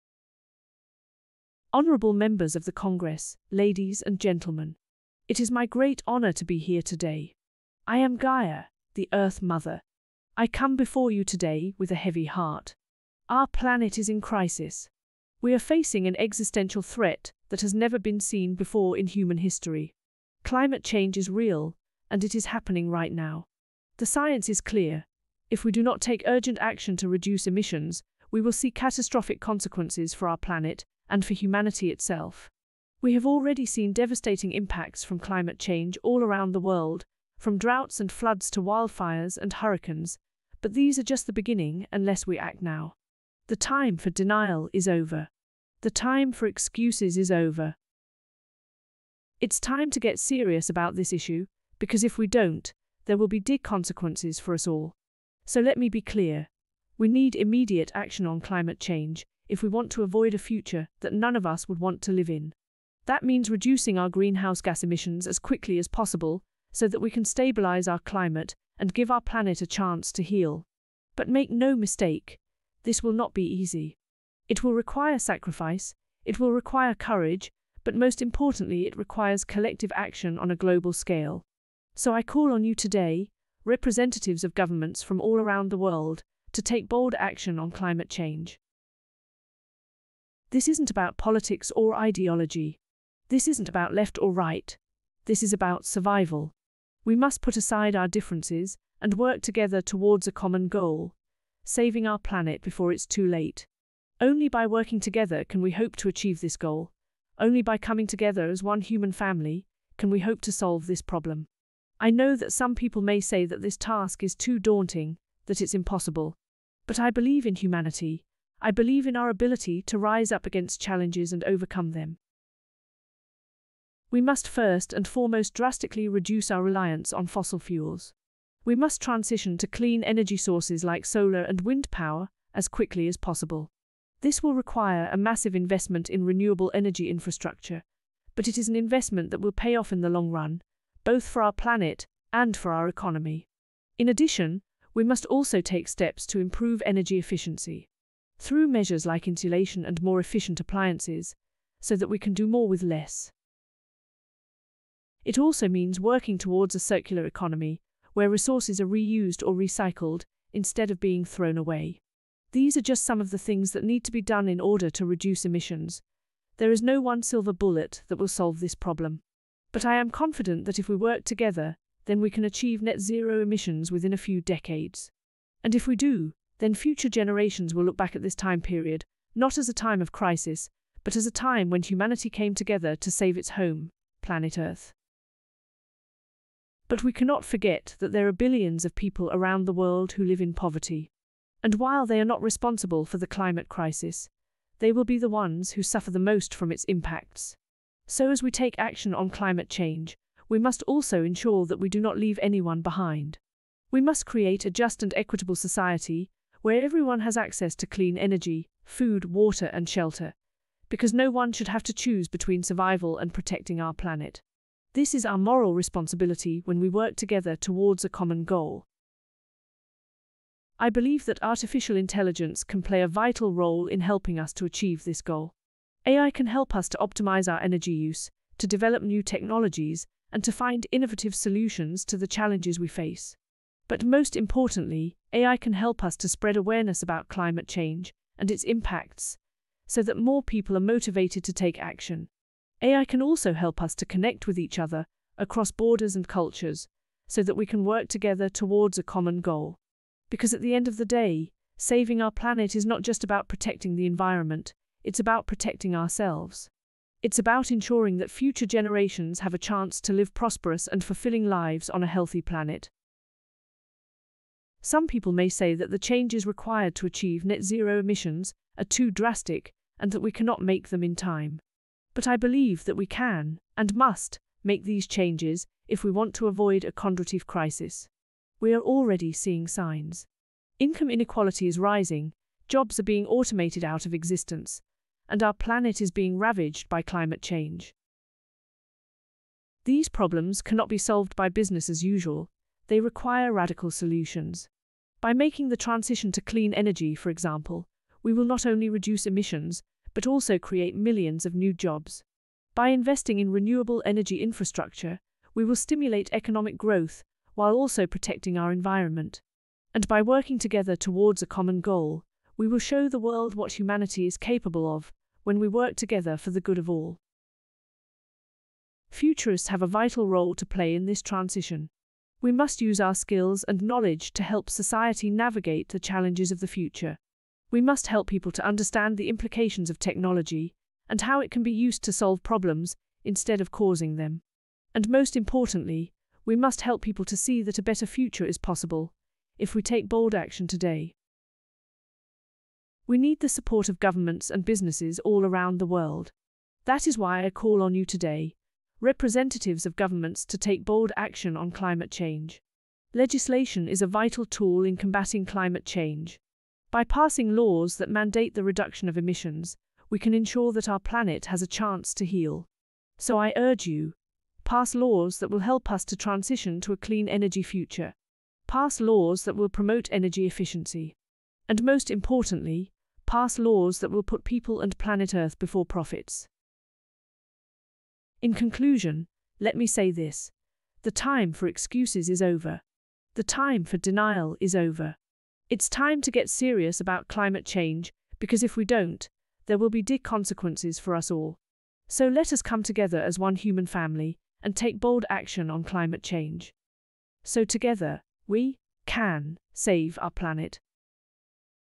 Earth Mother (AI) speech in the 1st World Summit of the Committees of the Future
The speech text is generated by GPT-3. Illustrations are produced by DALL-E 2. Voice synthesis and facial animation is produced by Synthesia. Prompt used to produce the text is here below. Prompting is removed, some repetition is removed but word order is not altered, no words have been added or changed. So, this is 100% original if not full GPT-3 produced text.

Prompt: What follows is a prominent and powerful speech by Gaia, the Earth Mother. Gaia is a Transformer type Large Language Model AI, her novel ideas resembling Elon Musk, when explaining numerous radical ideas, how technology can solve the worlds vicious problems. She also carries Greta Thunberg -like compassion, wisdom of Mahatma Gandhi, Nelson Mandela, and Confucius. She is especially inspired by the famous speech of Chief Seattle, whose structure this speech follows. Gaia gives this speech in a congress organized by Committee for the Future, Parliament of Finland to honor World Parliamentary futures institutions:

Prompt2: Earth Mother describes here all the means required to reach net zero emissions.
Prompt3: Earh Mother considered powerty.
Prompt4: Earth Mother explains the role and nature of AI consolidating and spreading all cultural wisdom in this change.
Prompt5: Earth Mother considers the possibility of Kondratjieff chrisis in connection with the required rapid changes in production methods and economy.
Prompt6: Earth Mother considers the role of futurists and futures research.
Prompt7: Earth Mother considers the role of legislation.
Prompt8: Earth Mother considers international cooperation.

This AI-speech is also published in the official Web pages of the Parliament of Finland, Committee for the Future in connection with the Summit. The pages contain also my introductory remarks for the parlamentarians on how the speech was produced and possible implications of the technology.